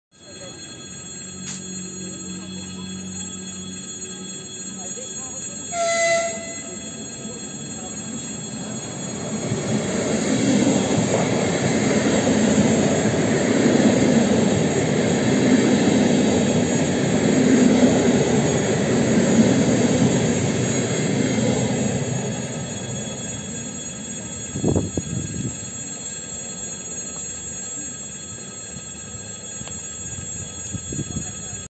Экспресс в Шереметьево подкрался незаметно
Снято на Савеловском направлении МЦД